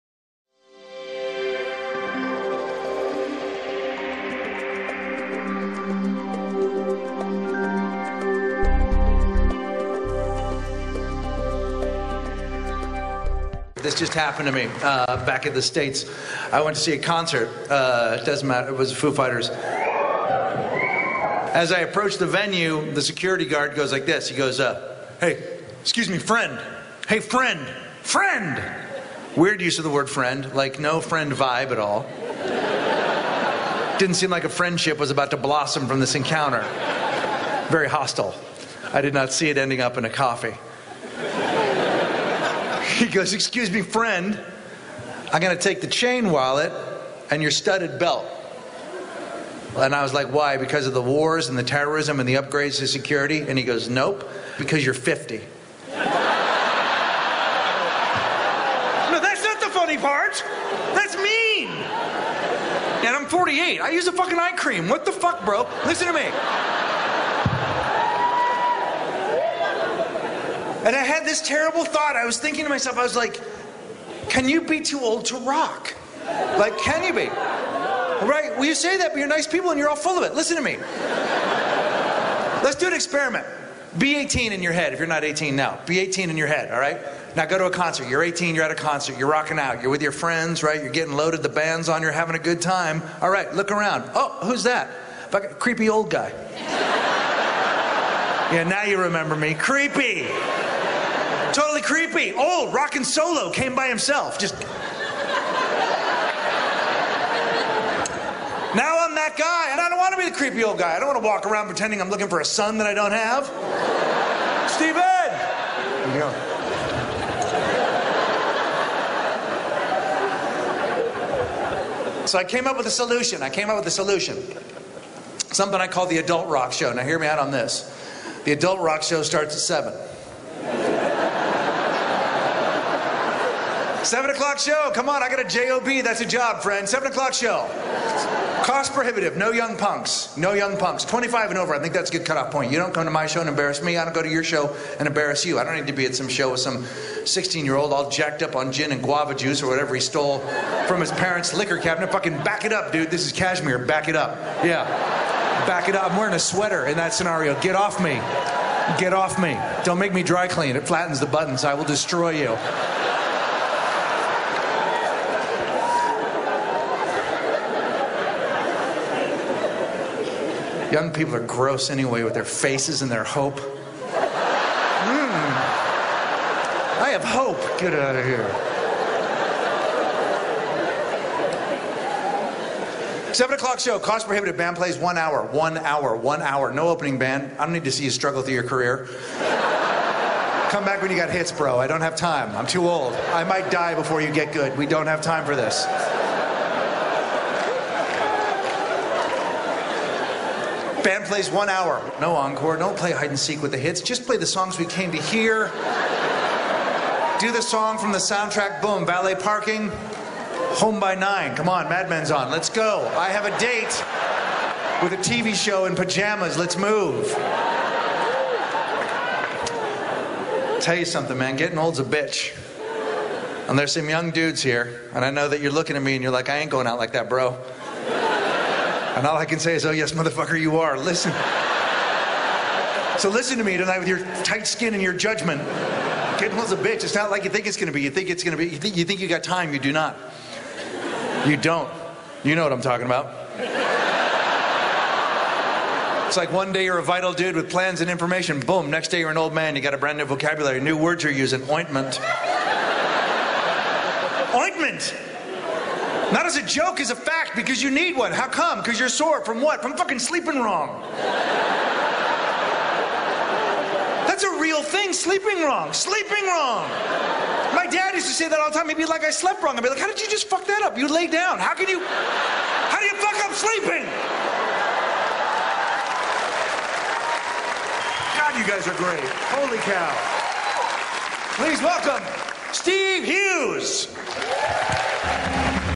Greg Behrendt on Getting Old
A well known stand-up comedian, Greg Behrendt quickly turned his keen eye on relationships and the mysteries of manhood into a gig as writer and consultant for Sex in the City. Greg Behrendt helped coin the phrase, "He's just not that into you," and turned that into a bestseller, titled, appropriately, He's Just Not That Into You, co-authored with Liz Tuccillo. Behrendt followed that up with It's Called a Break-Up Because It's Broken, co-authored with his wife, Amiira Ruotula-Behrendt. He's Just Not That Into You established Greg Behrendt as a voice of reason and inspiration for all of us negotiating - and sometimes drowning in - the treacherous waters of romantic relationships.
Greg Behrendt is a standup comic first and foremost. His comedy resume features HBO's Mantastic, Comedy Central Presents and Uncool, seen exclusively on Comedy Central, as well as many appearances on late night shows like The Tonight Show with Jay Leno, The Late Show with David Letterman and Late Night with Conan O'Brien. He was also named by Variety as one of the "10 Comics to Watch." Greg Behrendt also performs and records with his band, The Reigning Monarchs.